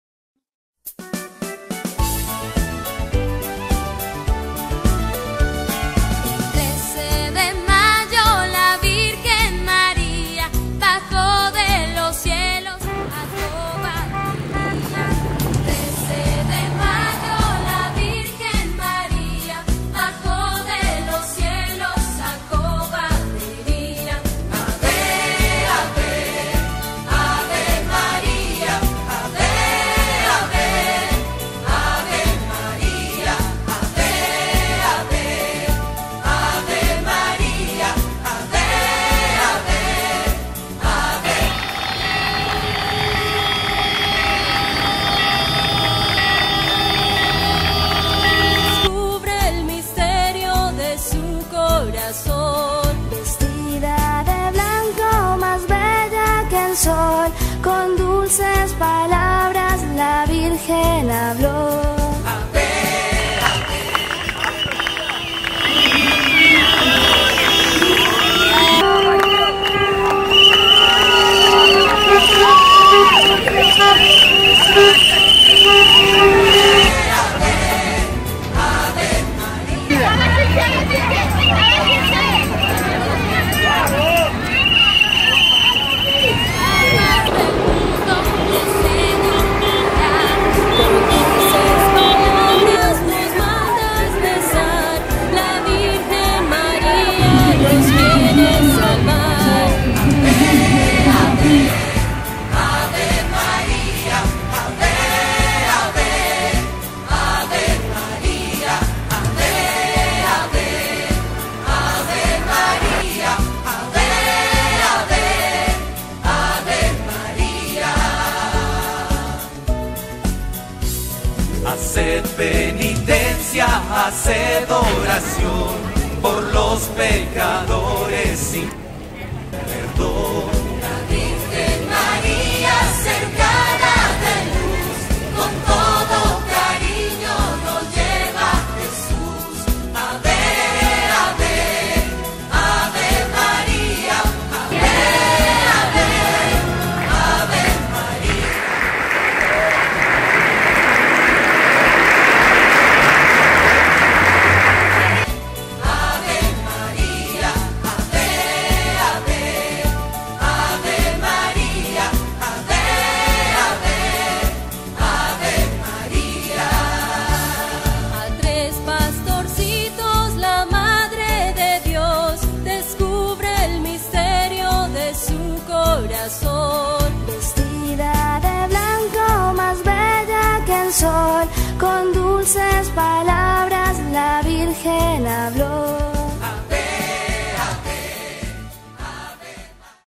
PROCESIÓN POR EL CENTENARIO DE LAS APARICIONES DE LA VIRGEN DE FÁTIMA
Con motivo de celebrar las Fiestas Patronales de las parroquias Virgen de Fátima de Cañuelas y de González Catán, se realizó la Peregrinación de la Virgen de Fátima, también patrona de nuestra Diócesis, comenzando en la Parroquia que lleva su nombre en el Km. 55 de la Ruta 3 en el partido de Cañuelas.
Pasando por el Barrio San Esteban, donde esperaban los chicos del Grupo Scout Nuestra Señora de Fátima, que también se sumaron, luego en el Km 41 se detuvieron en la Comunidad Parroquial Cristo Rey, ya en la localidad de Virrey del Pino, en el Km 39 la comunidad del Santuario Nuestra Señora de la Esperanza recibió la procesión con mucha alegría... Cuatro kilómetros más adelante la Comunidad Nuestra Señora de Lourdes esperaba con banderas y carteles el paso de la Virgen...

A la llegada en González Catán, celebró la Misa Mons. Gabriel Barba, junto con el párroco, P. Marcelo Genovese, P. Fabián Urueña, párroco en Fátima de km 55 y Vicario General de la Diócesis junto con sacerdotes acompañantes.

Felicitamos a las parroquias por el gran trabajo realizado en la procesión y en las Fiestas Patronales. Una verdadera fiesta.